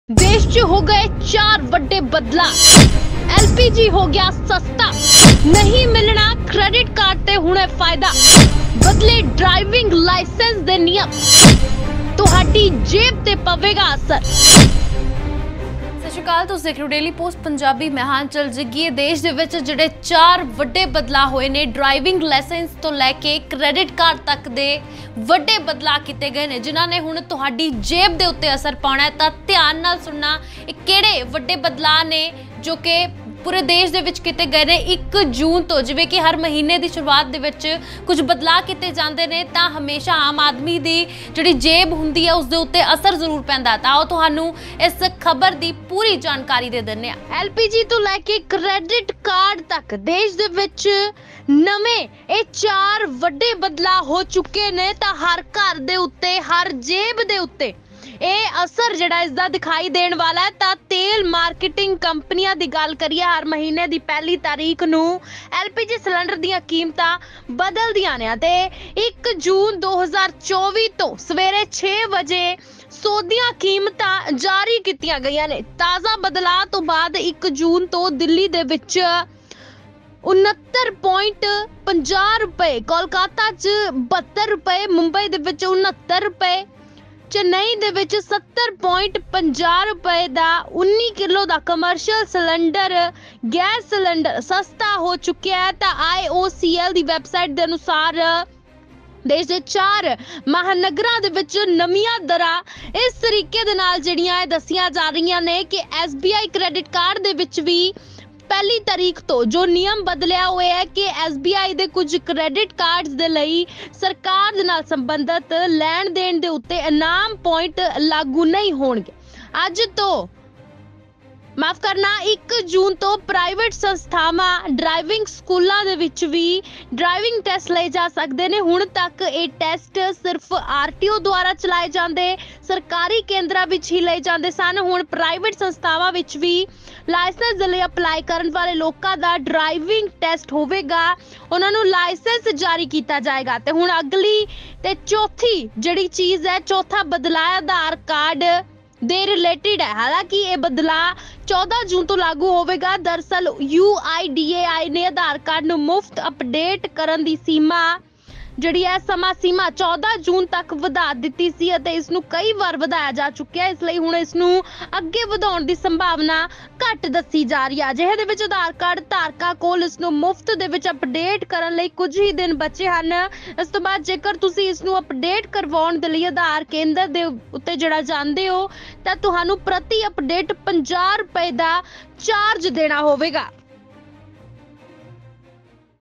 LPG ਹੋ ਗਿਆ ਸਸਤਾ, ਨਹੀਂ ਮਿਲਣਾ Credit Card 'ਤੇ ਹੁਣ ਇਹ ਫਾਇਦਾ, ਬਦਲੇ ਡ੍ਰਾਈਵਿੰਗ ਲਾਇਸੈਂਸ ਦੇ ਨਿਯਮ
LPG ਹੋ ਗਿਆ ਸਸਤਾ, ਨਹੀਂ ਮਿਲਣਾ Credit Card 'ਤੇ ਹੁਣ ਇਹ ਫਾਇਦਾ, ਬਦਲੇ ਡ੍ਰਾਈਵਿੰਗ ਲਾਇਸੈਂਸ ਦੇ ਨਿਯਮ, ਦੇਸ਼ 'ਚ ਹੋ ਗਏ ਵੱਡੇ ਬਦਲਾਅ, ਤੁਹਾਡੀ ਜੇਬ੍ਹ 'ਤੇ ਪੈਣਾ ਅਸਰ